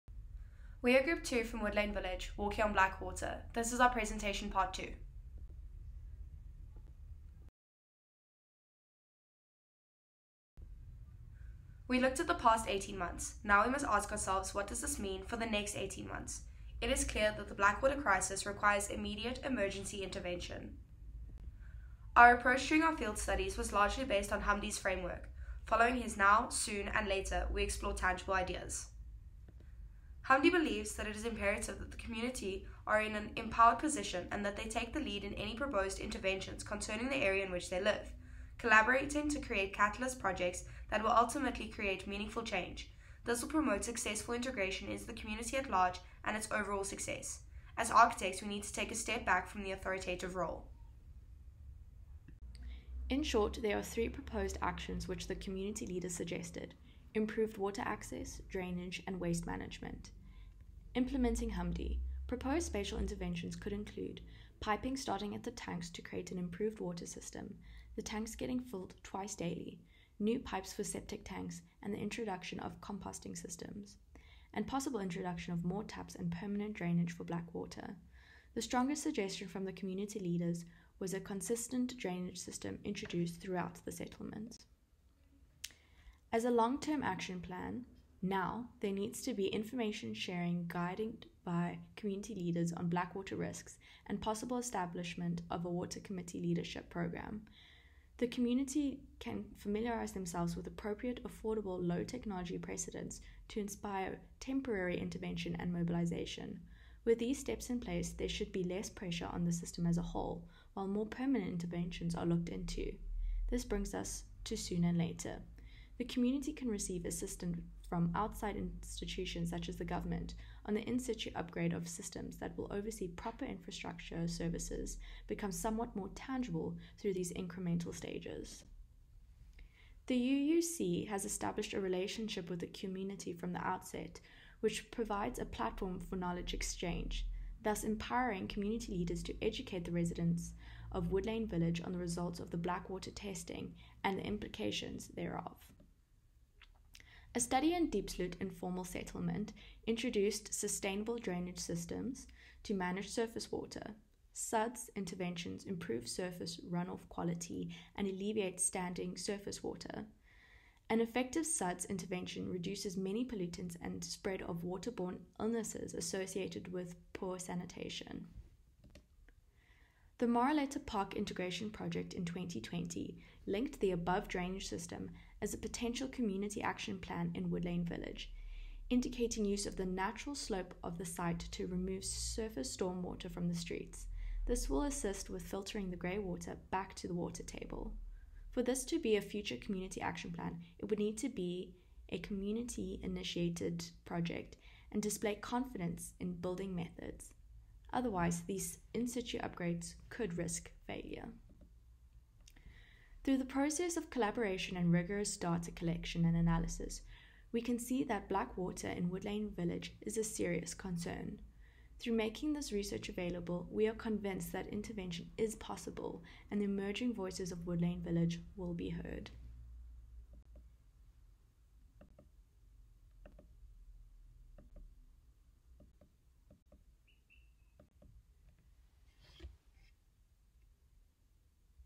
Urban Citizen Studio - Woodlane Village / Plastic View informal settlement - Part 4 Blackwater
This narrated presentation is from the work submitted by the Honours students at the Department of Architecture, University of Pretoria, South Africa. The work was done in collaboration with Masters students from the Reality Studio at Chalmers University of Technology, Sweden.

The research study aimed at understanding the challenges and strengths of the communities of Melusi and Plastic View informal settlements (Woodlane Village) through participatory action research. Both of these settlements are situated in Pretoria, South Africa.

A team of multi-disciplinary researchers from the University of Pretoria is conducting a 3-year (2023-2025) study on co-creating wellness and human dignity in informal settlements. The purpose of this study is to document and map the community services, buildings, environment and to understand living conditions in these communities and how this affects health and well-being.

During March 2023 the researchers mapped the settlements and conducted voluntary questionnaires within the community. The findings were presented to the community and stakeholders to enable the development of action plans in the future.